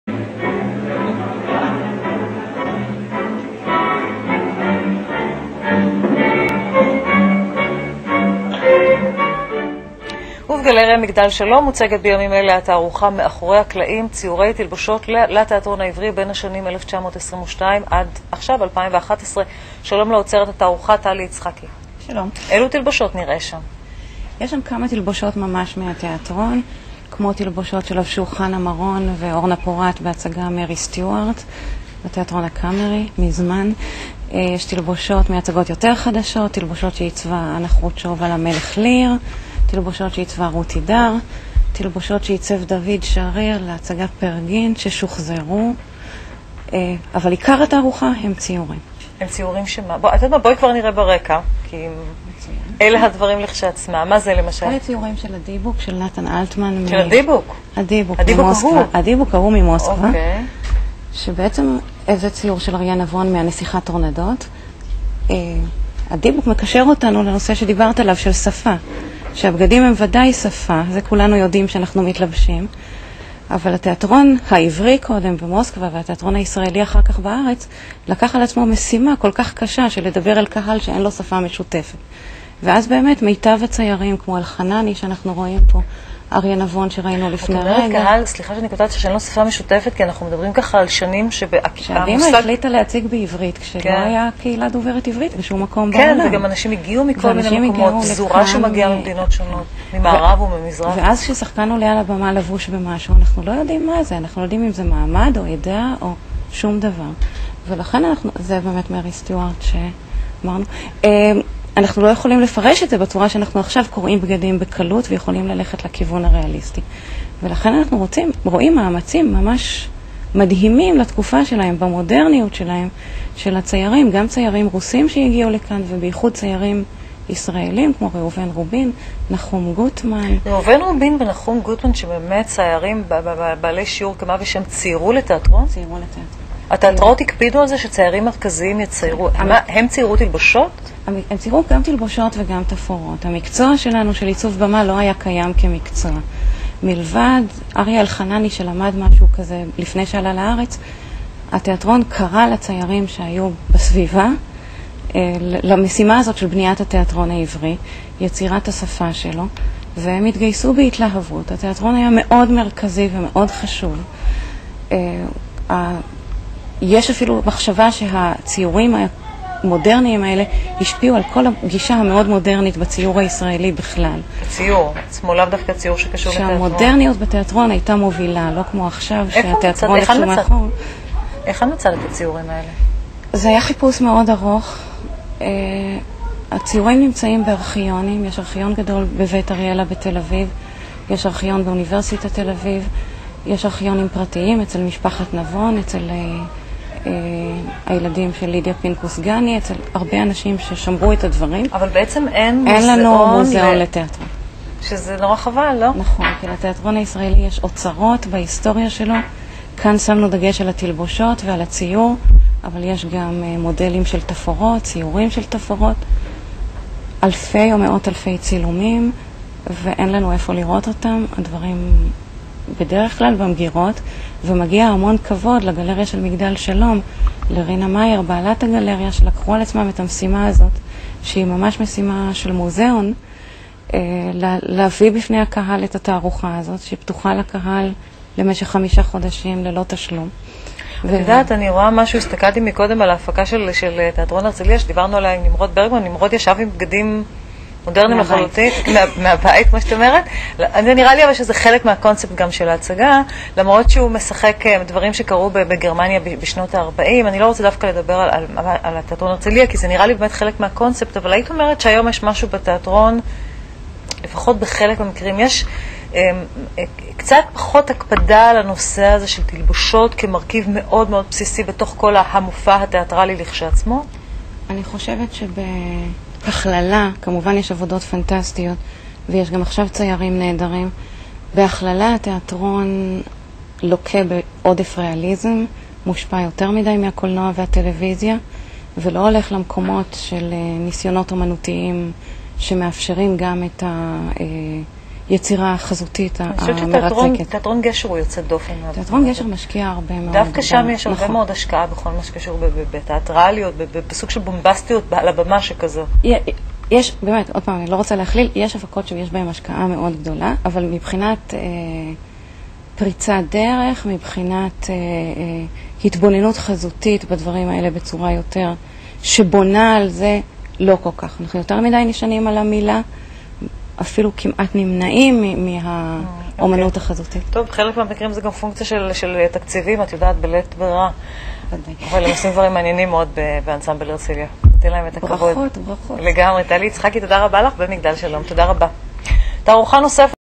Costume Exhibition Tal Itzhaki 18.1.11.rm
Curator Tal Itzhaki interviewed on Channel 1 of the Israel TV about the  exhibition "Behind the Scenes,  Costumes on the Hebrew Stage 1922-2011 at the Shalom Tower Gallery, Tel Aviv
האוצרת טל יצחקי מרואיינת בערוץ הראשון של הטלויזיה הישראלית על התערוכה "מאחורי הקלעים", ציורי תלבושות תיאטרון על הבמה העברית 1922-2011, בגלריה מגדל שלום, תל אביב